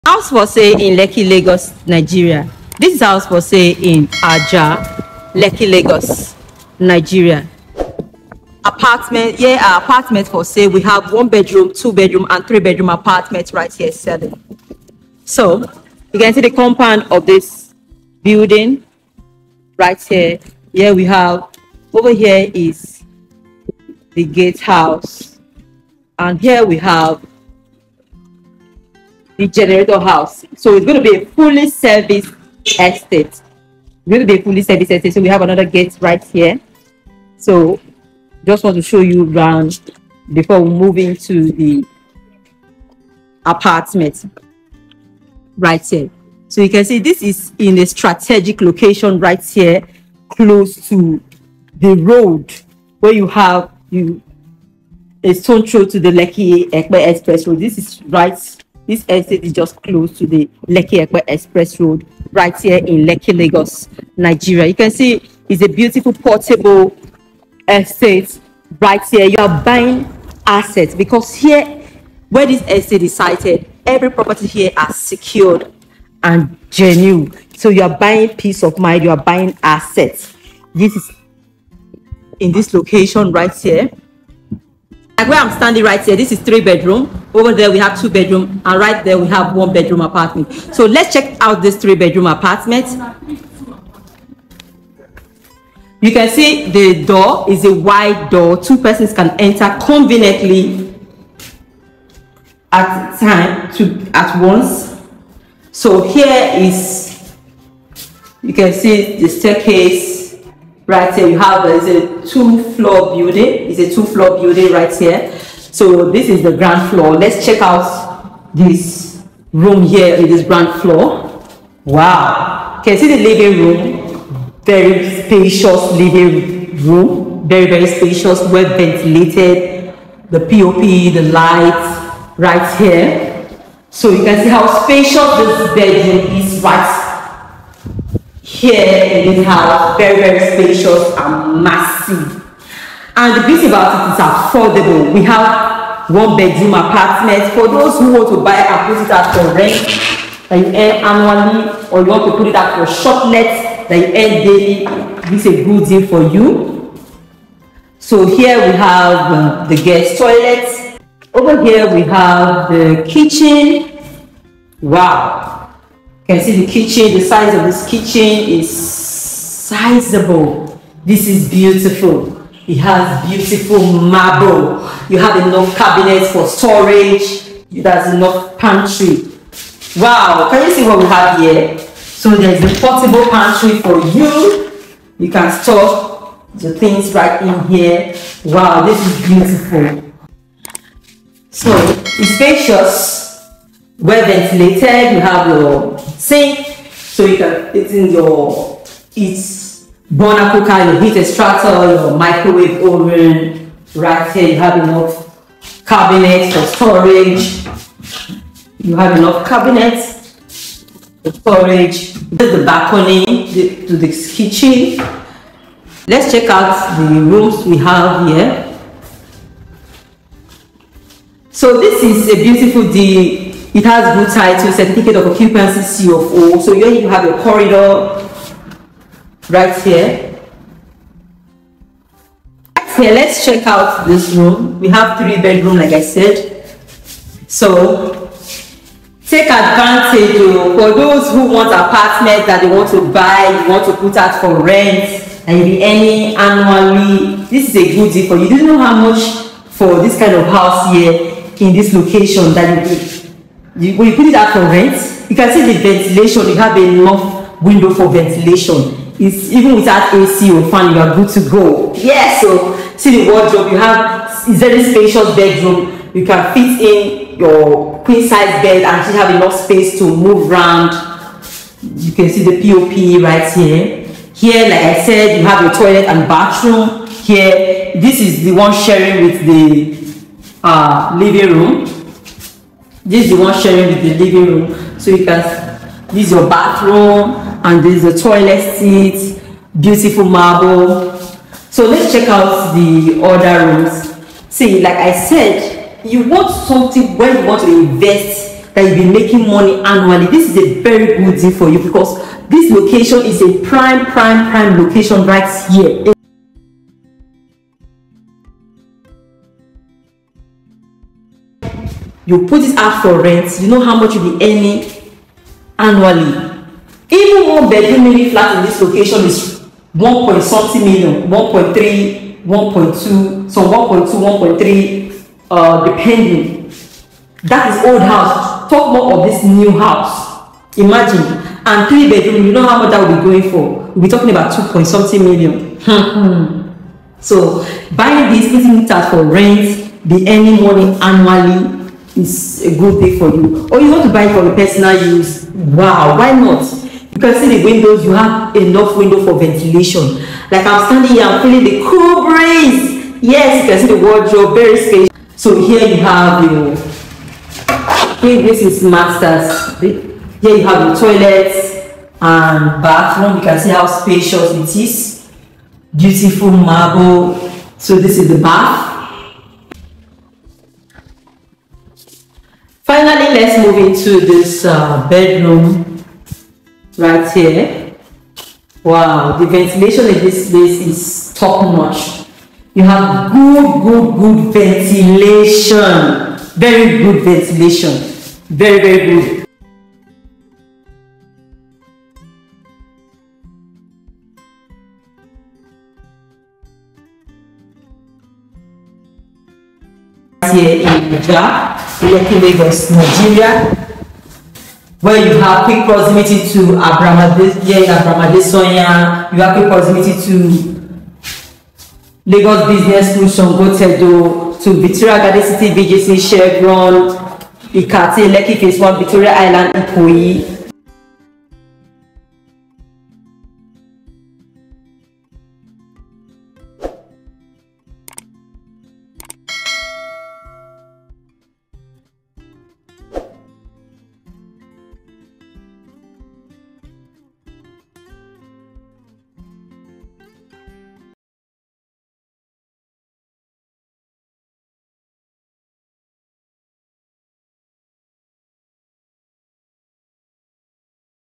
House for sale in lekki Lagos Nigeria: Luxury 1, 2 & 3 bedroom apartment in Ajah lekki 35m
Location: Ajah Lekki

FEATURES

Brand new luxury home
Spacious Living room
bedroom
kitchen
Pantry
Ample parking
All room en-suite
Serviced apartment